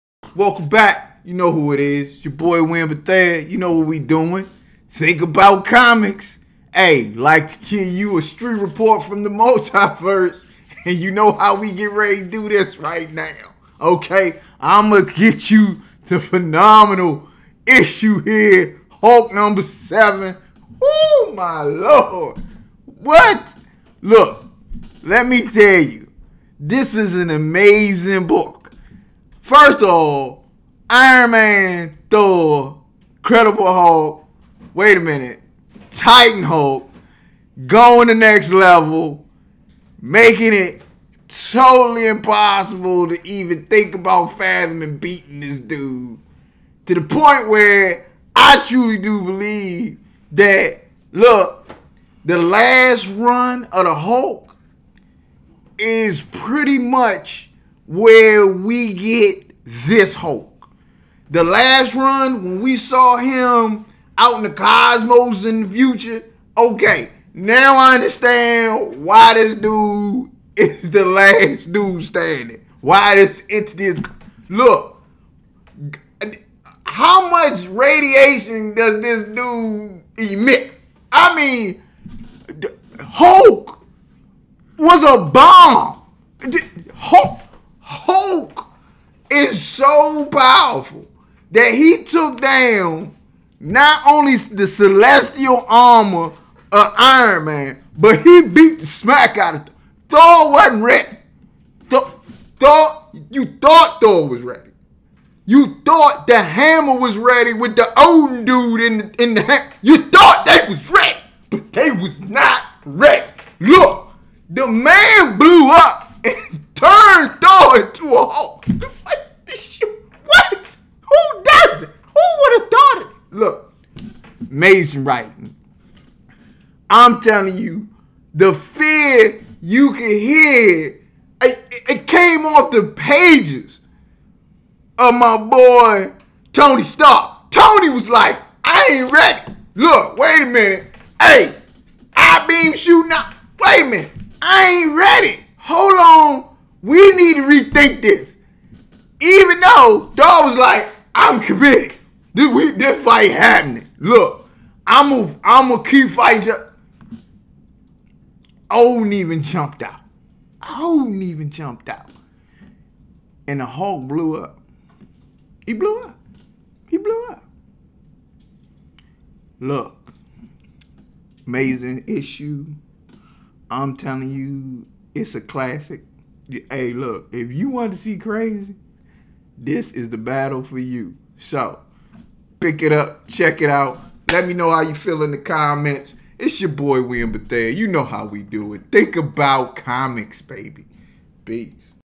Hulk 7 Review
Bethea reviews Cates's run on the Hulk. This series is doing everything in its power to be completely different from immortal Hulk.